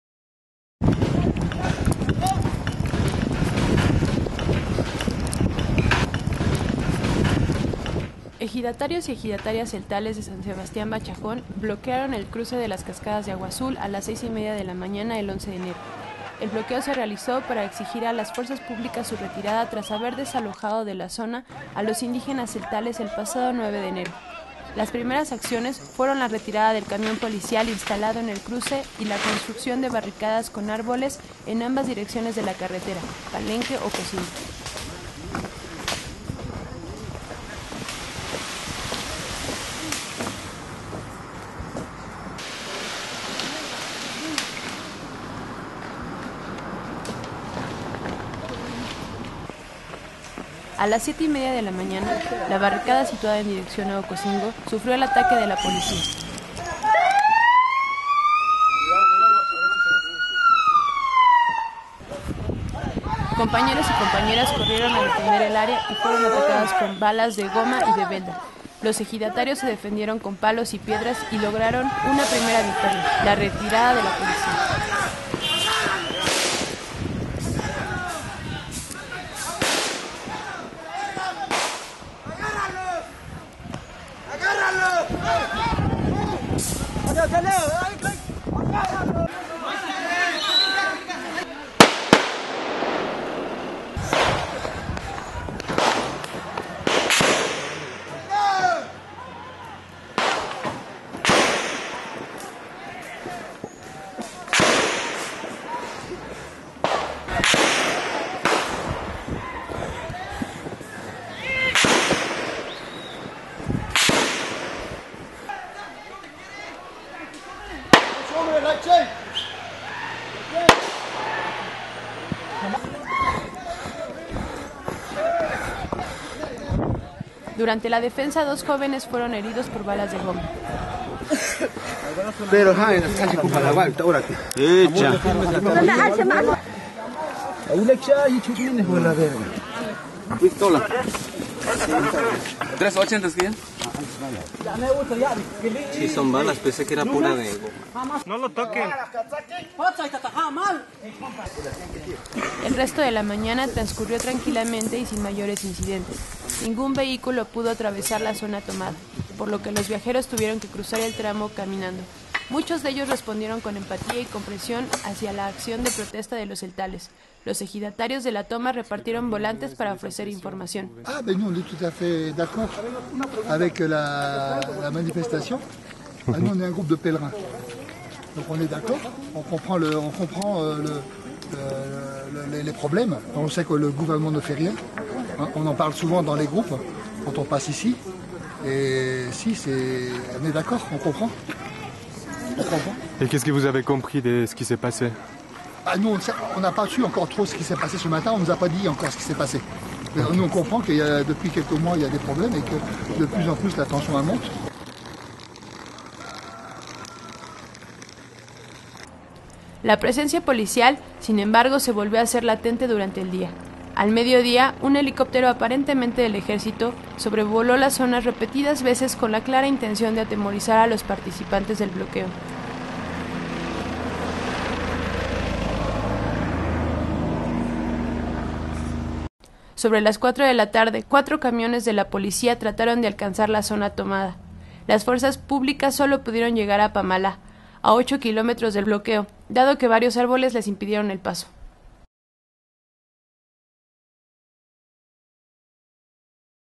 Crónica de una resistencia tseltal
Crónica de las primeras horas del bloqueo en la carretera Palenque-Ocosingo, a la altura del cruce del poblado Agua Azul, en resistencia contra el despojo de las tierras en el ejido San Sebastián Bachajón.